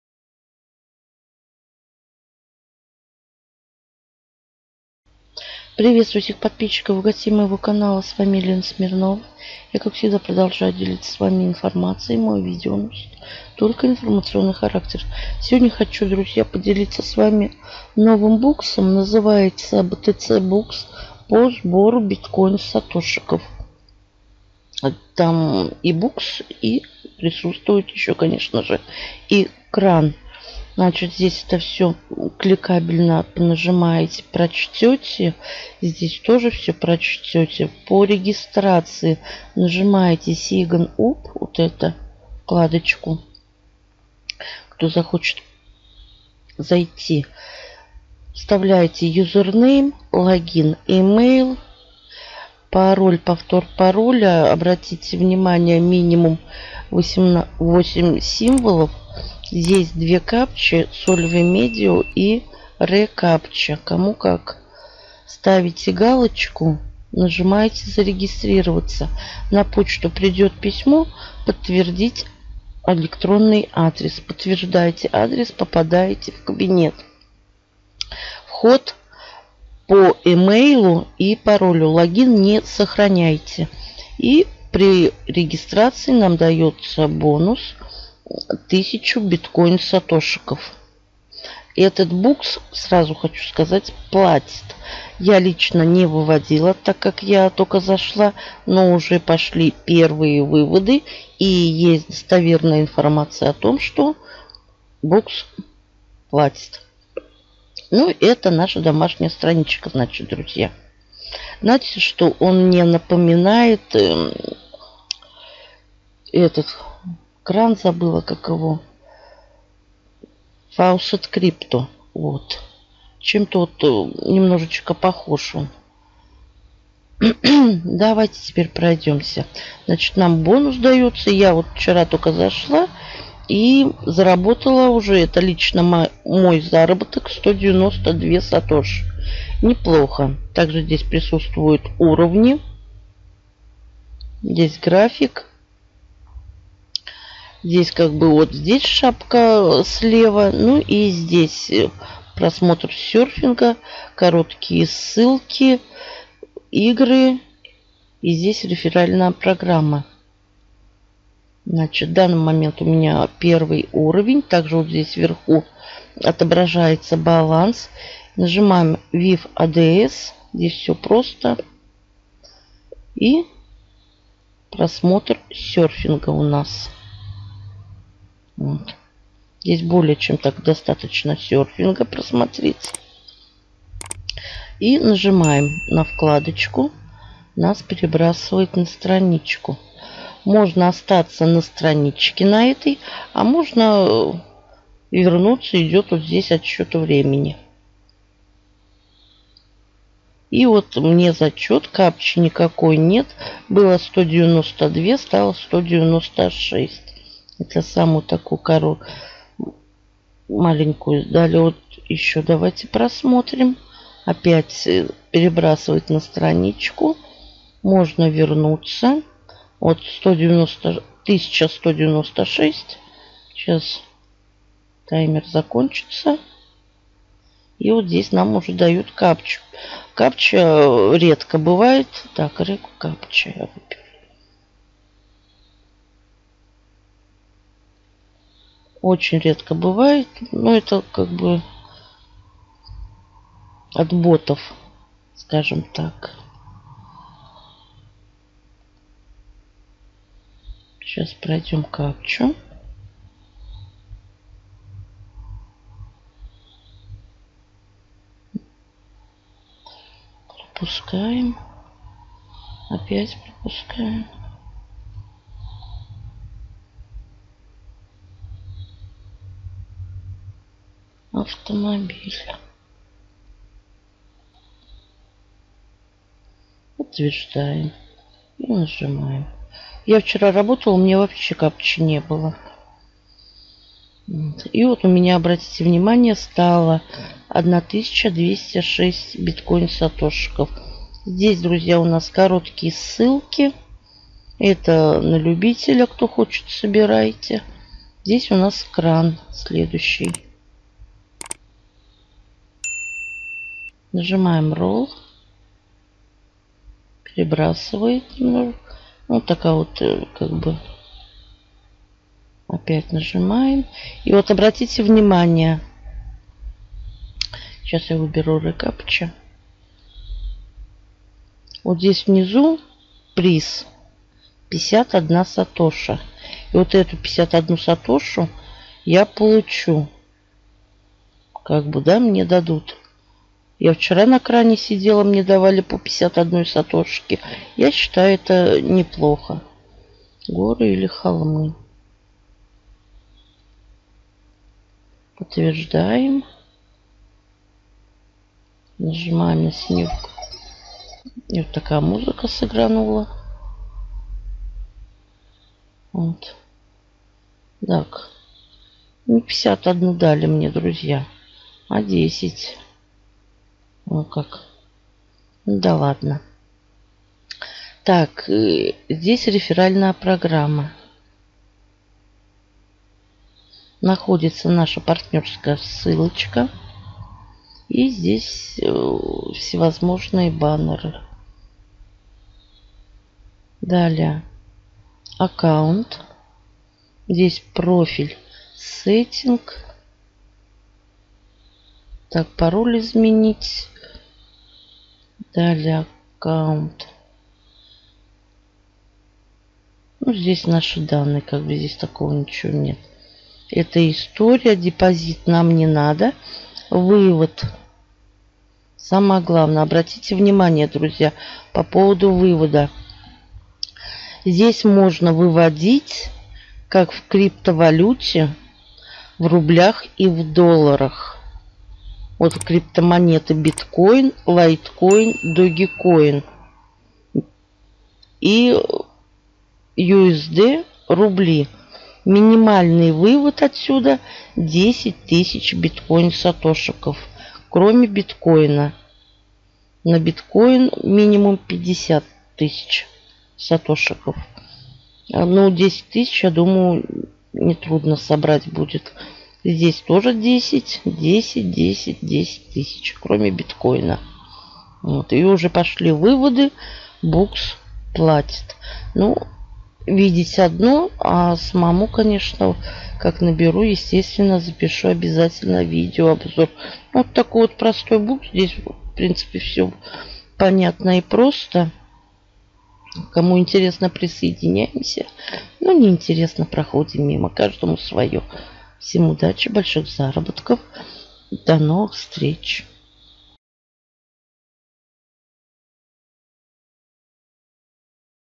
Без вложений НОВИНКА! BTCBux , вывод в крипомонетах, рублях, долларах
регистрация
бонус за регистрацию 1000 SATOSHIS
КОШЕЛЁК payeer.
Perfect Money (Перфект Мани).
обменник Magnetic Exchange
регистр.
постепенно будут добавляться новые краны.